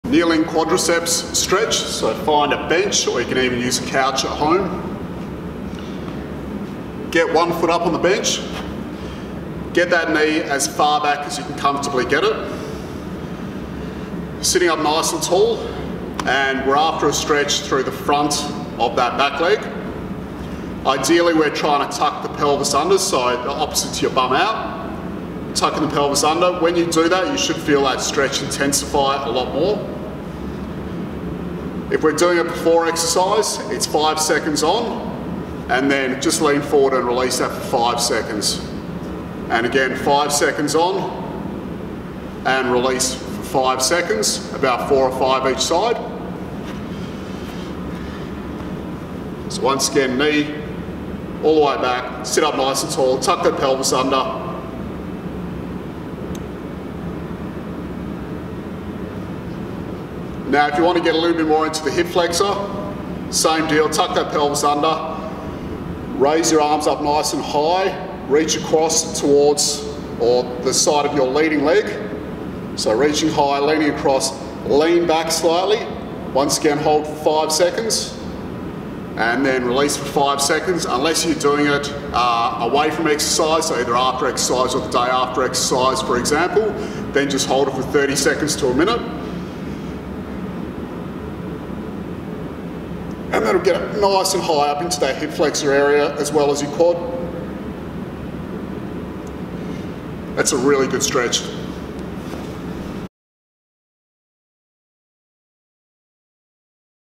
Quad Stretch To Improve Hip Mobility
Get better movement through your hips and release lower back tightness with this great stretch!